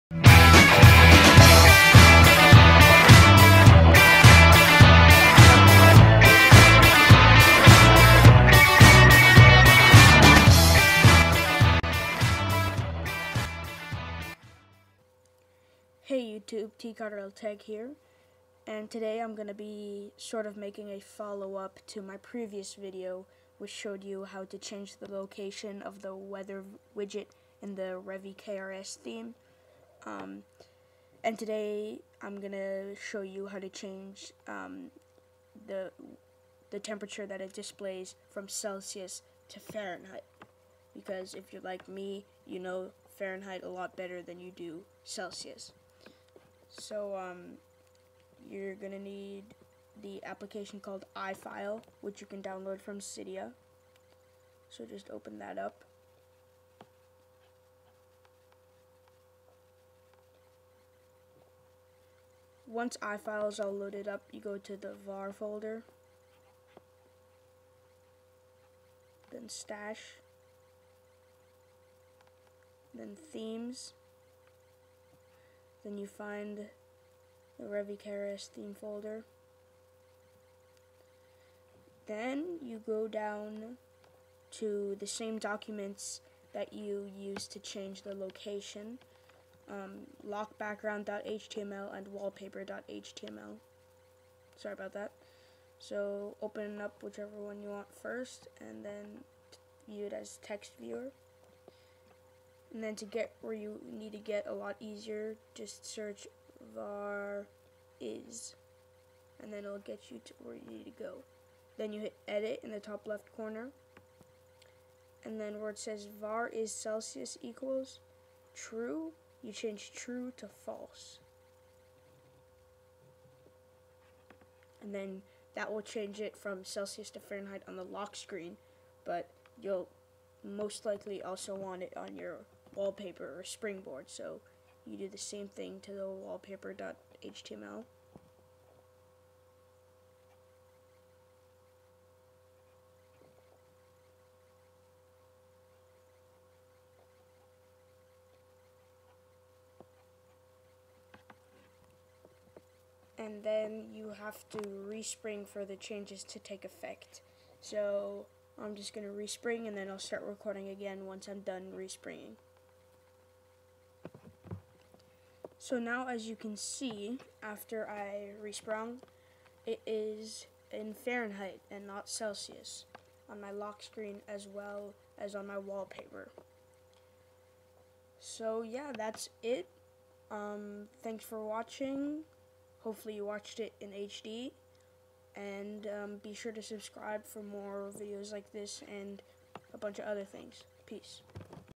How to Change the Temperature from Celsius to Fahrenheit in Revi-Krs
If you have this theme, you most likely have noticed that the weather widget displays the temperature in Celsius. In this video, I teach you how to change it to Fahrenheit.